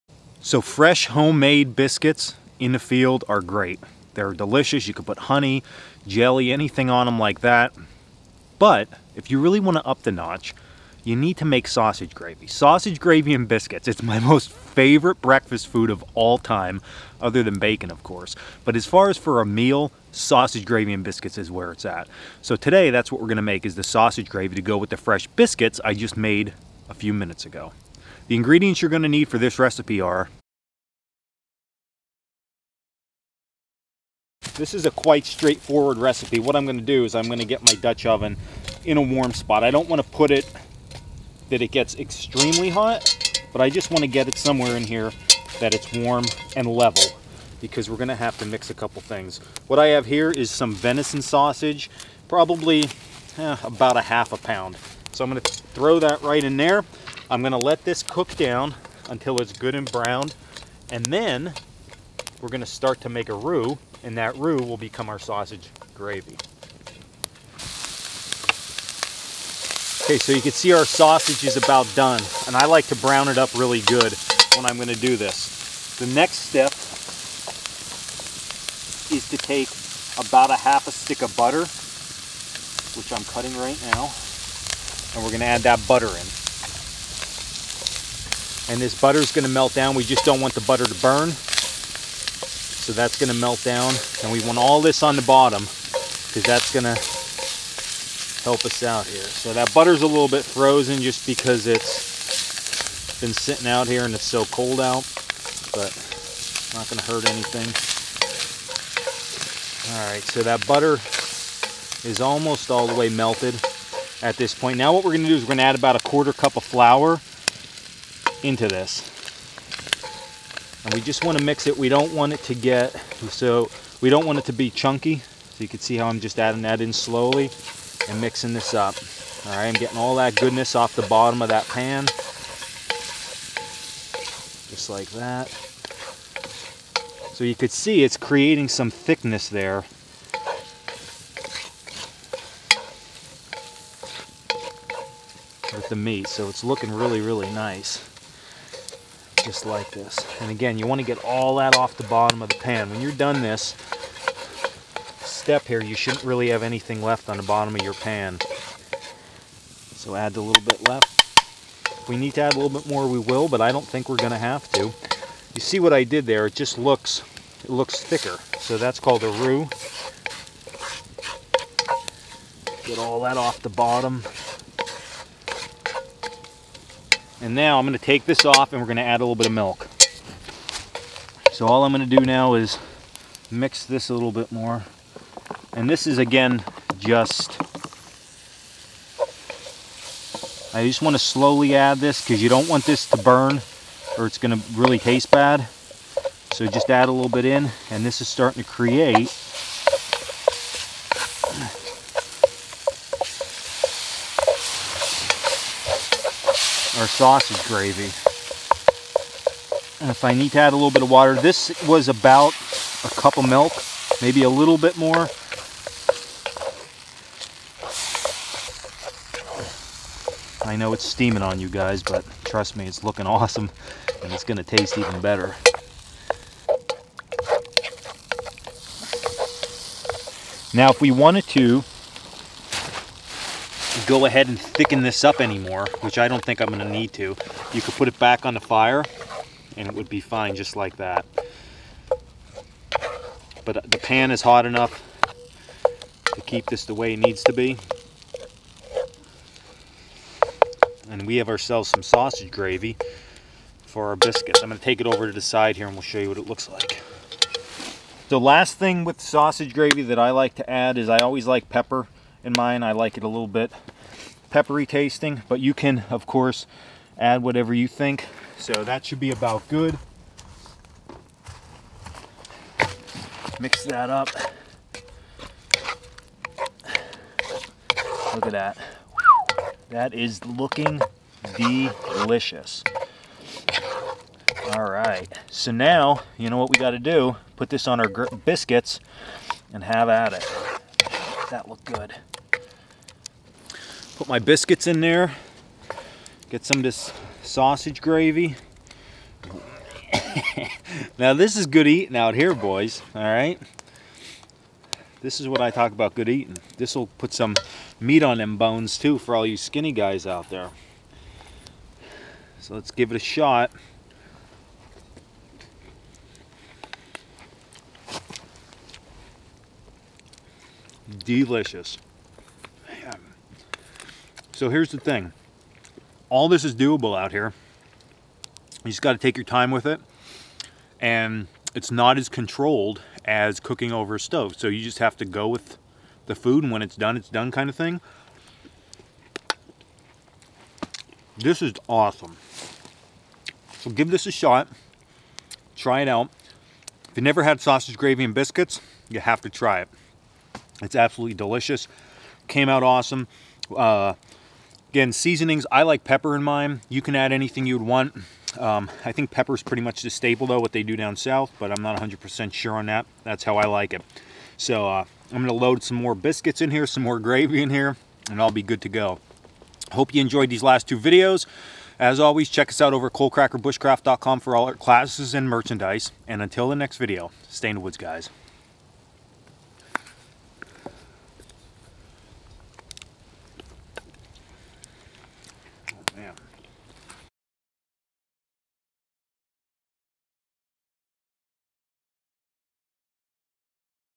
Cookin' with Coalcracker: Sausage Gravy
Sausage Gravy over fresh biscuits is one of my most favorite breakfast foods. In this episode we take a look at how to make sausage gravy in the field